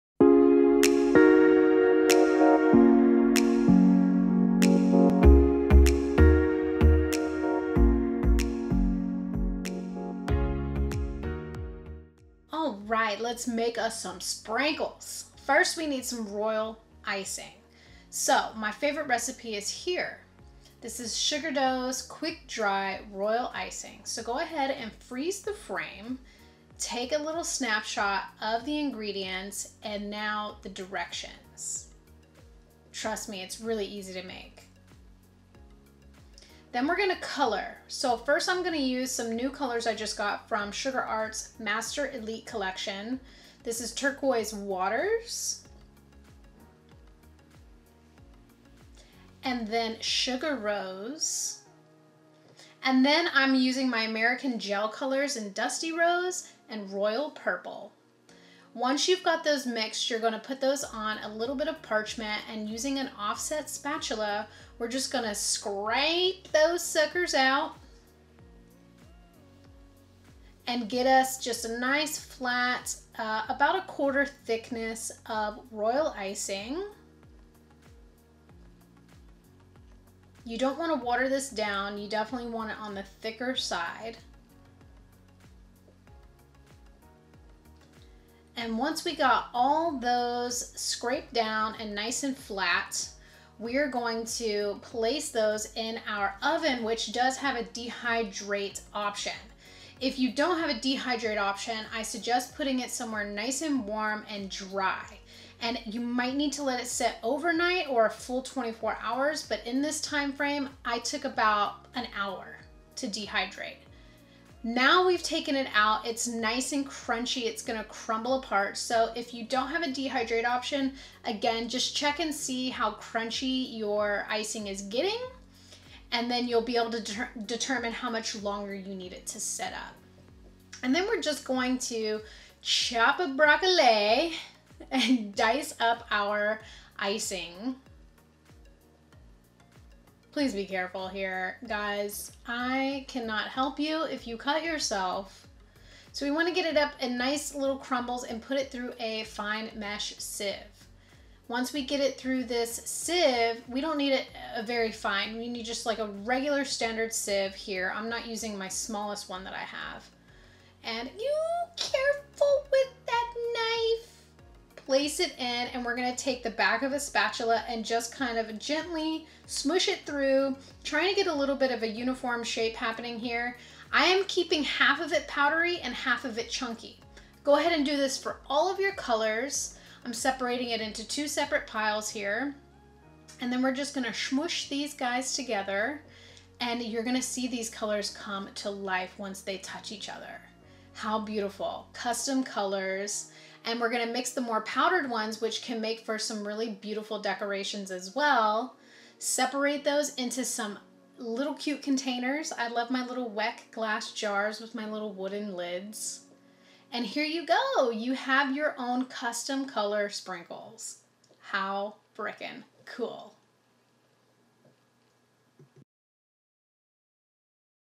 How to make Sprinkles
Here is quick and easy to make sprinkle video to compliment our delicious homemade poptarts (coming soon).
Sprinkles can be expensive and hard to find in just the right combo of colors. So if you're looking to make your own awesome combo of colored sprinkles, look no further!

XOXOXOXOXO
BisouxBakery.com